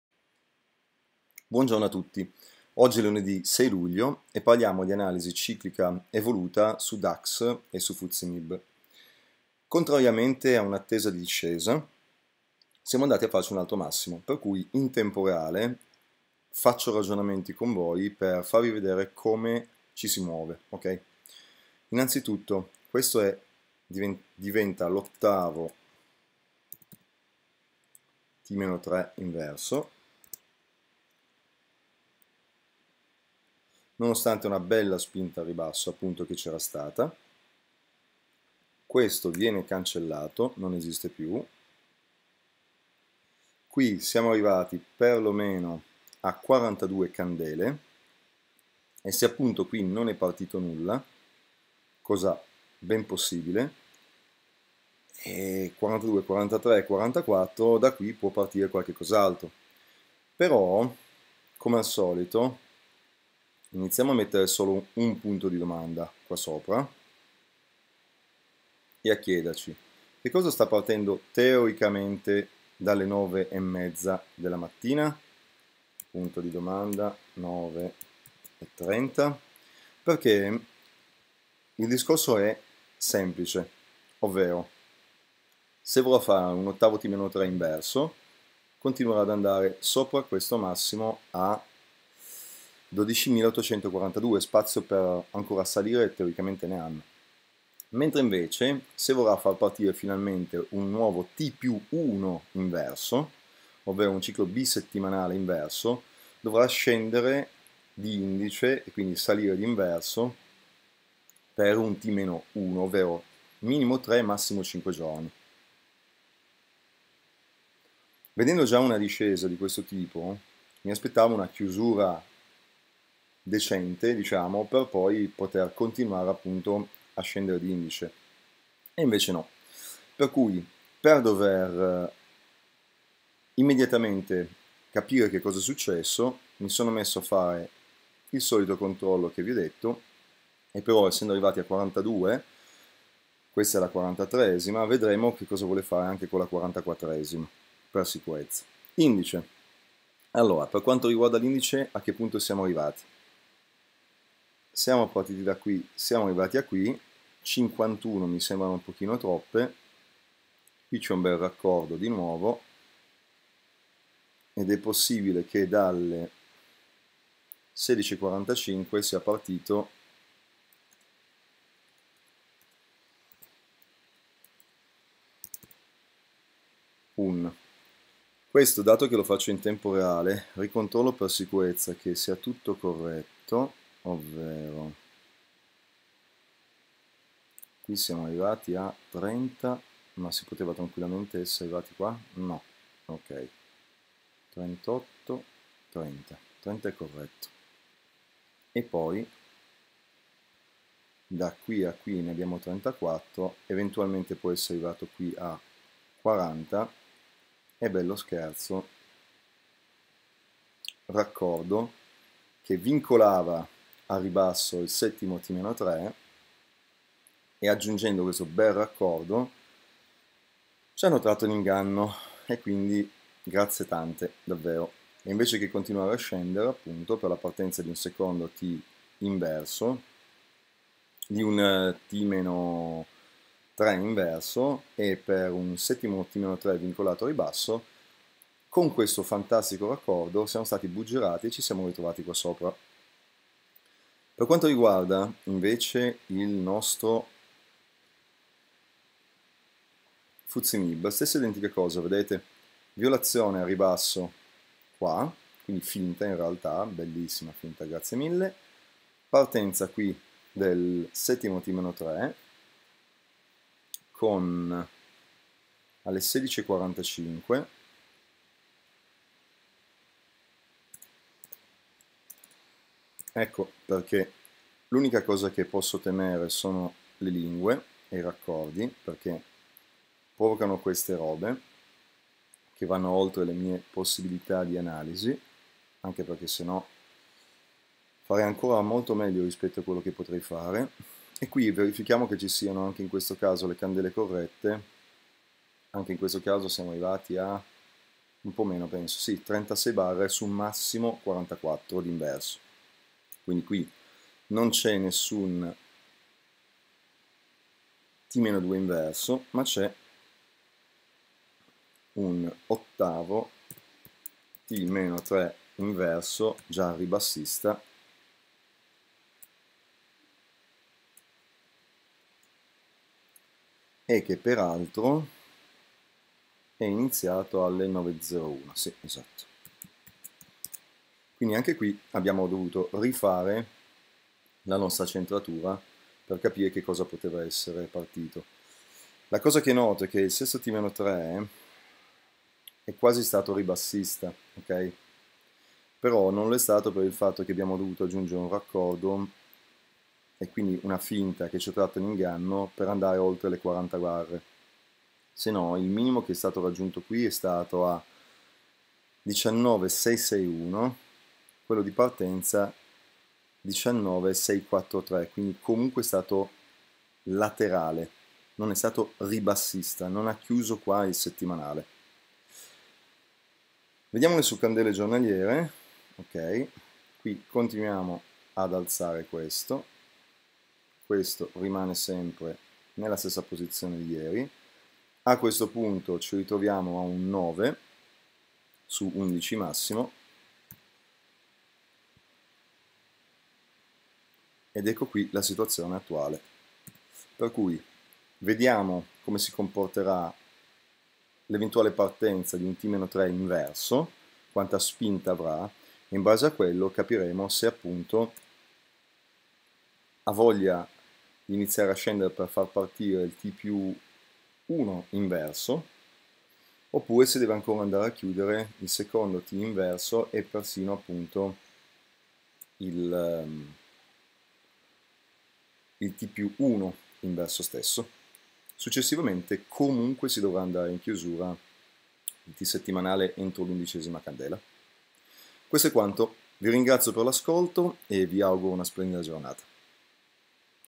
DAX + FTSE Mib - Analisi Evoluta di lun 6/07 - Il raccordo ciclico che svia
Buongiorno,

Cosa è andato storto rispetto all'analisi settimanale di ieri?
Un semplice raccordo ciclico. Pensavamo a un 7'T-3 già vincolato a ribasso e invece... era una finta. Oggi partiamo con uno svantaggio anche se il T+1i inverso può raggiungere al massimo gli 11 giorni.

Buon ascolto.

Jacopo